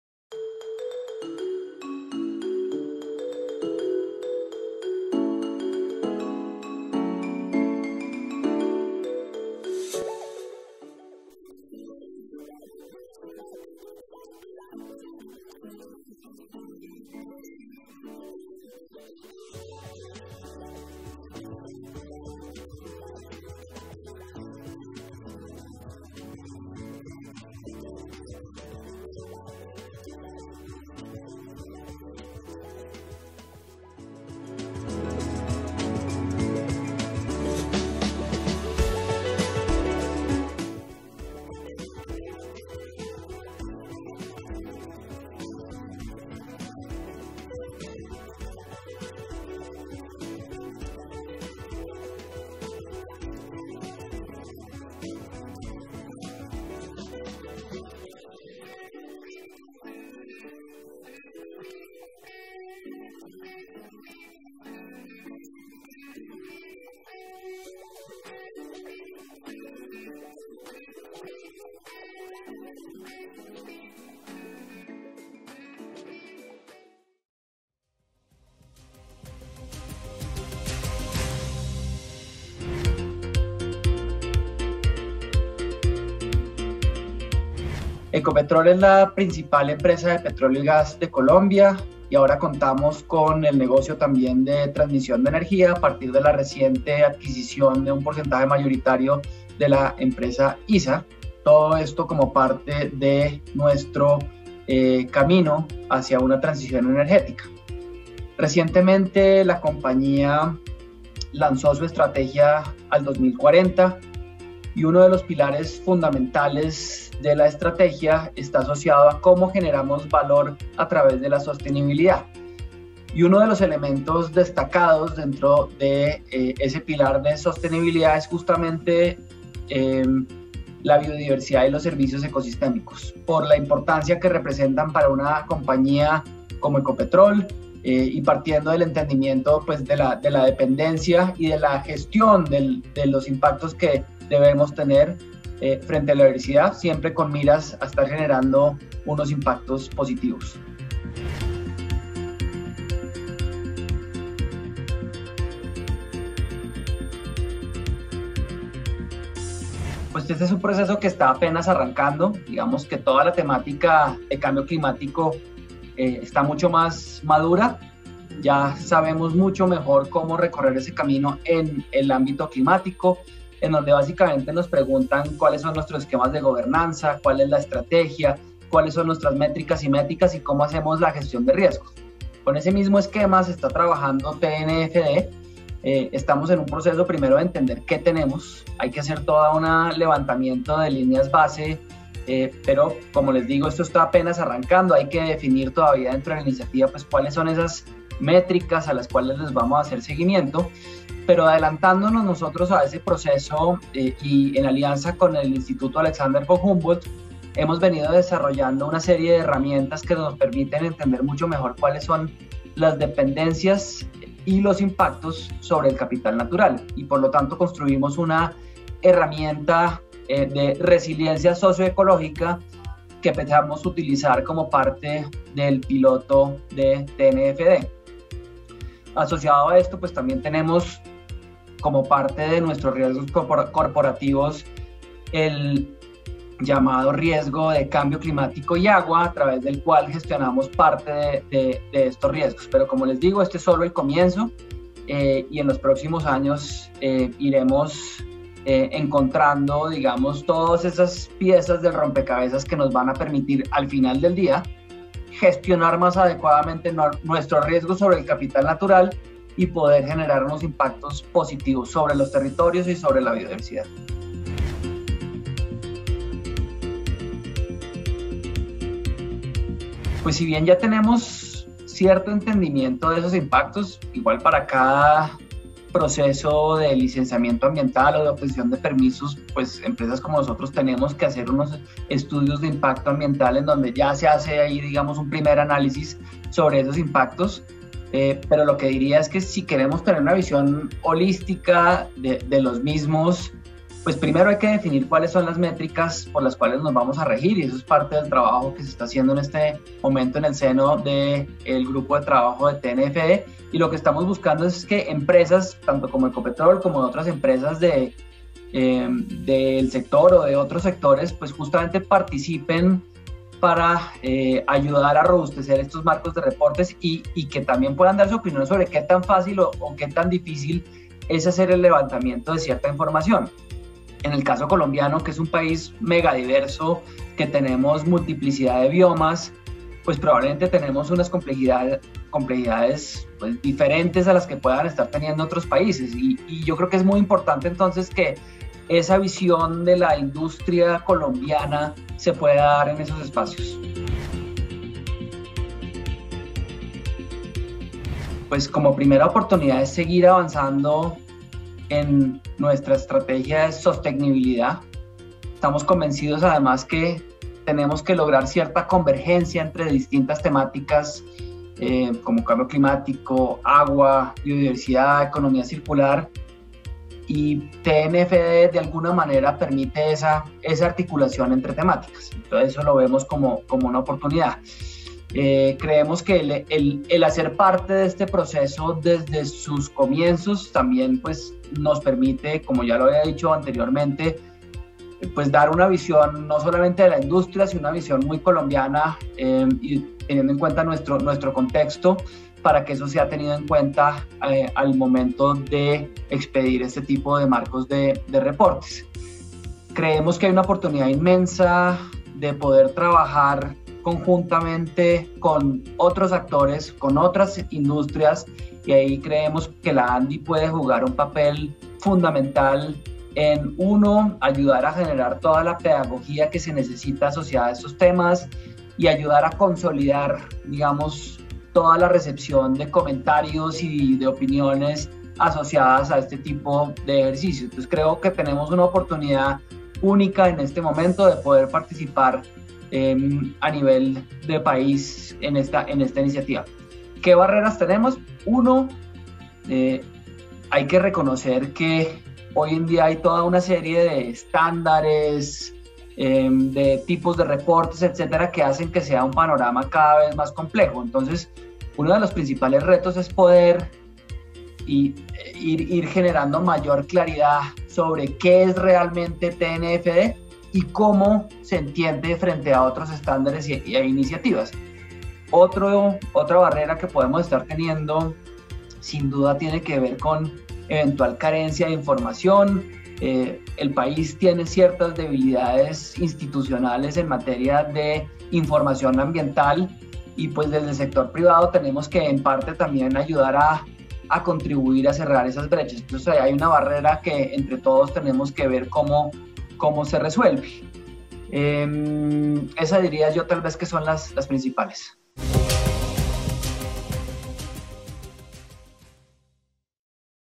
ANDI - BfN. Voces frente a como evaluar y medir dependencias e impactos a la naturaleza
ANDI - BfN. Voces frente a como evaluar y medir dependencias e impactos a la naturaleza por parte de empresas y sector financiero.